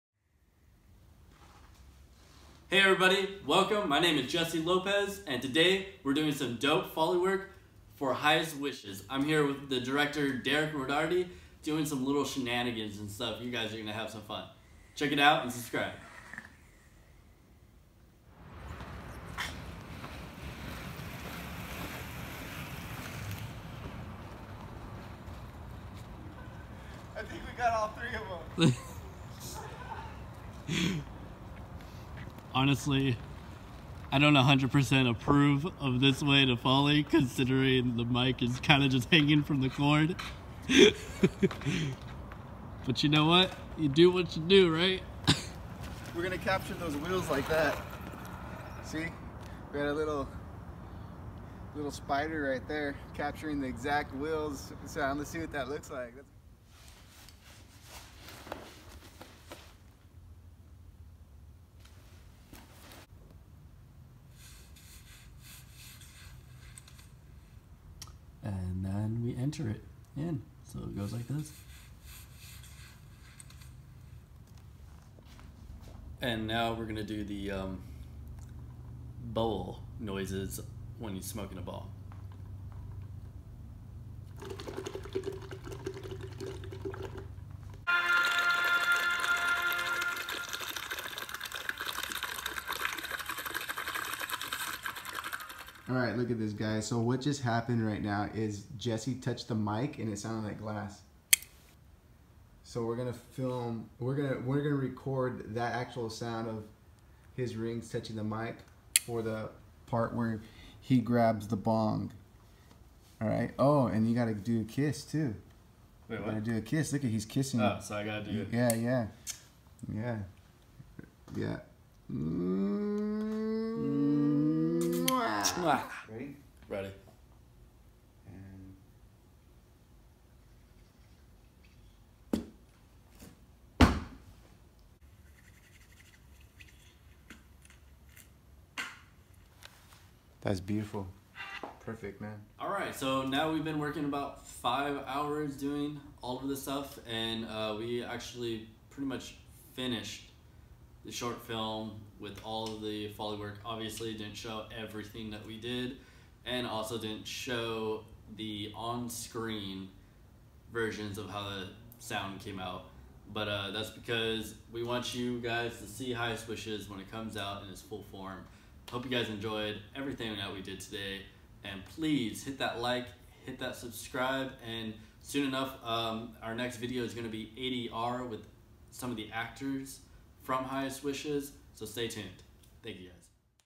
Fun Folly Night
Another late night at the studio doing Folly work.